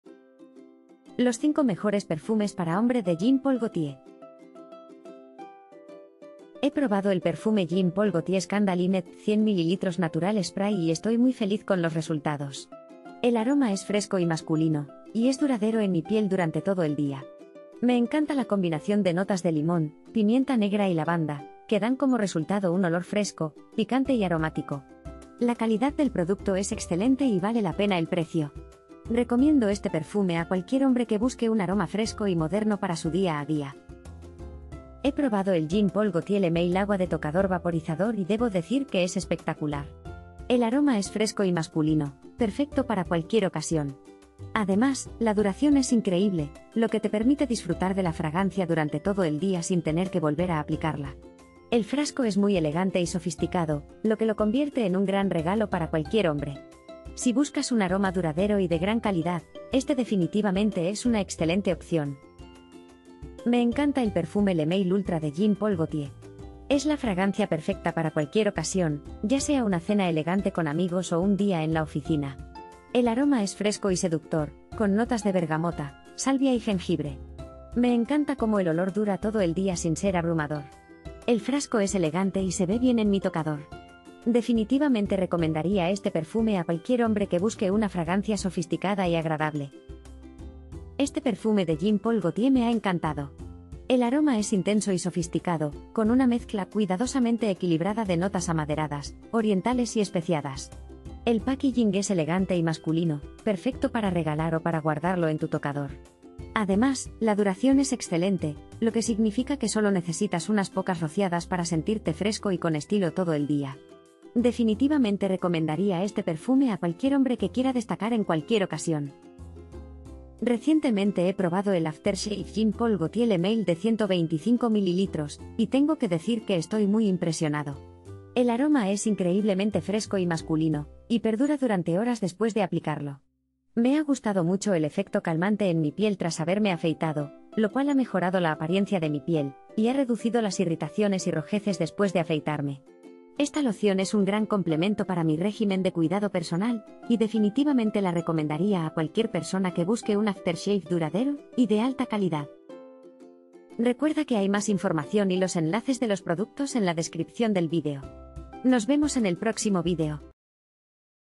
Jean paul gaultier hombre: Los 5 mejores perfumes para hombre de Jean Paul Gaultier
✅ Más información

✋  Aquí están los 5 mejores perfumes para hombre de jean paul gaultier disponibles en el mercado.

🔽🔽🔽 ENLACES DE COMPRA JEAN PAUL GAULTIER HOMBRE 🔽🔽🔽

1) JEAN PAUL GAULTIER SCANDAL HIM EDT 100 ML NATURAL SPRAY

2) Jean Paul Gaultier Le Male Agua de Tocador Vaporizador - 75 ml

3) Jean Paul Gaultier de Toilette Le Mâle Ultra 200 ml

4) JEAN PAUL GAULTIER Jpg Le Male Edp 125V Exc. 125 ml

5) Jean Paul Gaultier Le Male After Shave 125 ml

00:00 - Intro
00:05 - 1) JEAN PAUL GAULTIER SCANDAL HIM EDT 100 ML...
00:33 - 2) Jean Paul Gaultier Le Male Agua de Tocador...
01:02 - 3) Jean Paul Gaultier de Toilette Le Mâle Ultra...
01:29 - 4) JEAN PAUL GAULTIER Jpg Le Male Edp 125V...
01:59 - 5) Jean Paul Gaultier Le Male After Shave 125...
02:32 - Outro